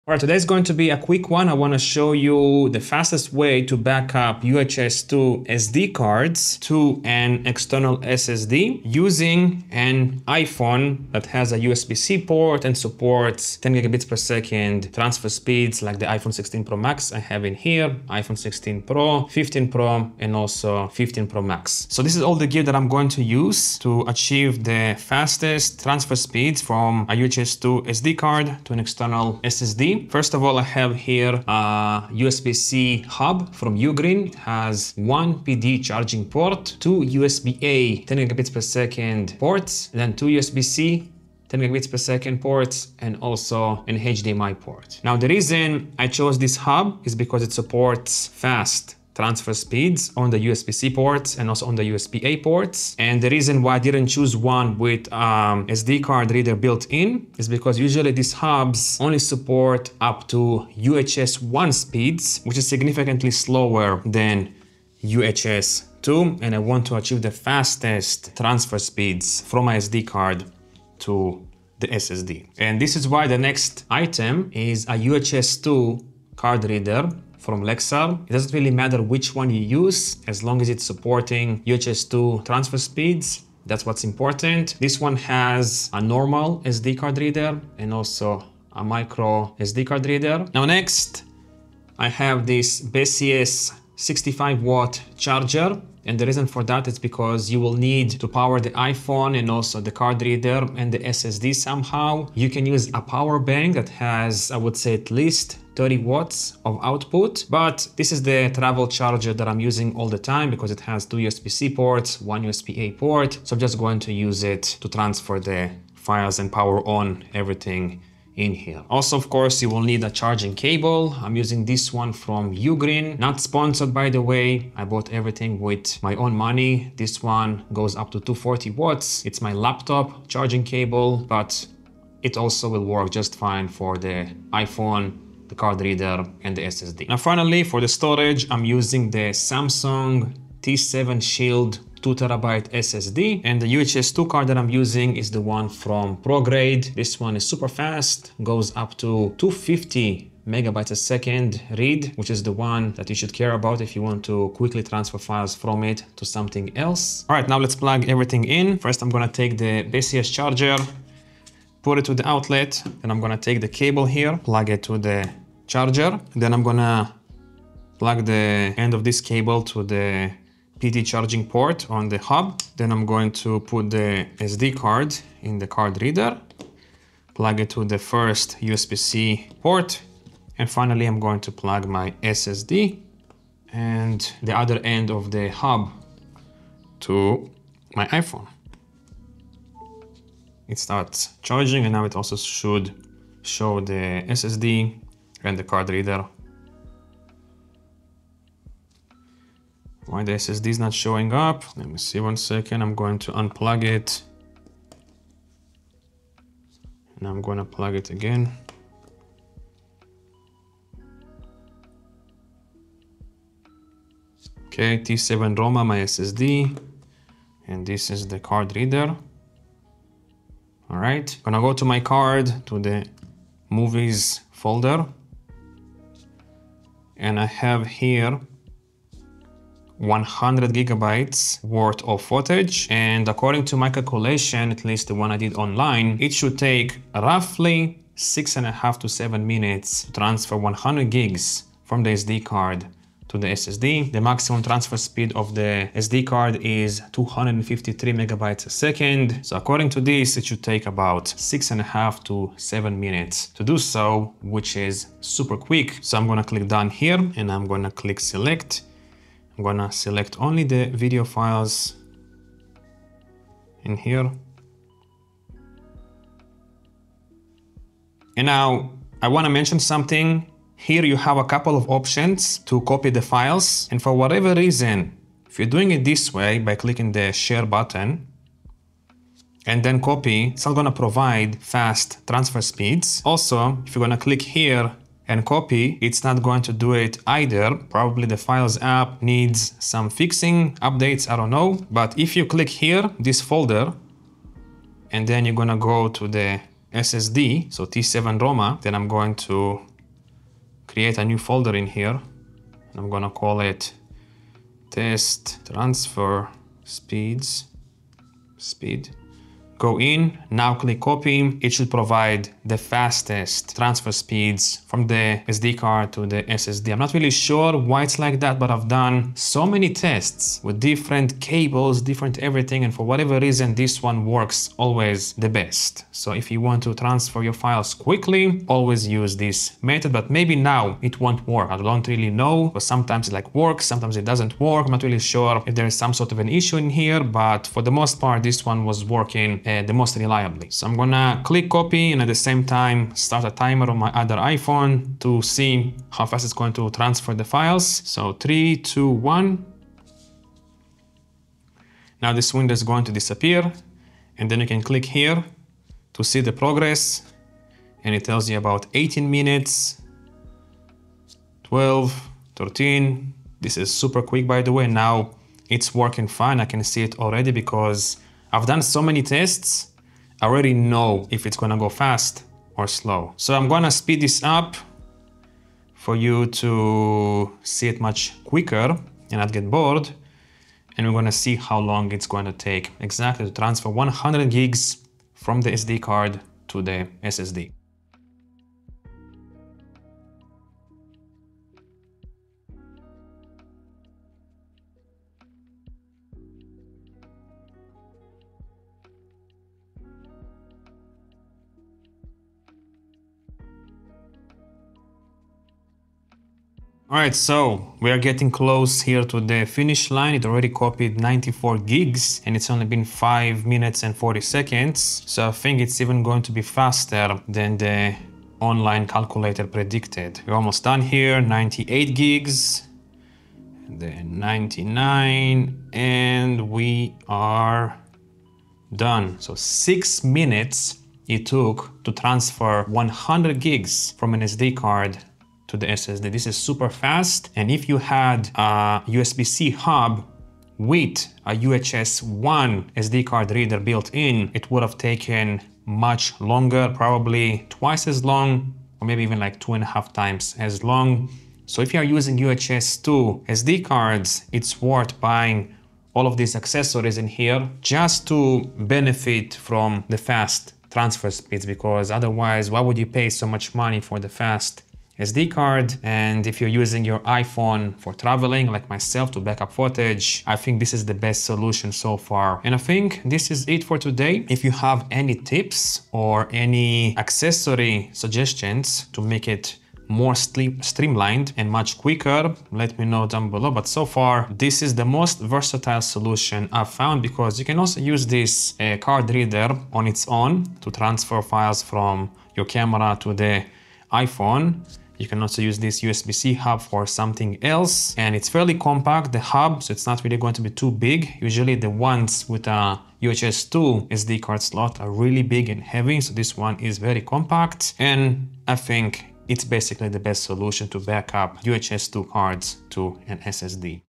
Back up UHS-II SD Cards to a SSD - iPhone 16 Pro
Music & SFX I use:
Artlist (sign up to get 2 months free)

My camera
Zoom lens
Prime lens
ND Filter
Pocket camera
EDC bag
Key light
Microphone

0:00 - Intro
0:30 - USB-C hub
1:12 - SD card reader
1:30 - USB-C charger
1:58 - USB-C cable
2:16 - External SSD
2:23 - UHS-II SD card
2:36 - Plugging in everything
3:41 - Files app
4:28 - Important Files app tip!
5:48 - 100GB transfer speed test
7:17 - Conclusions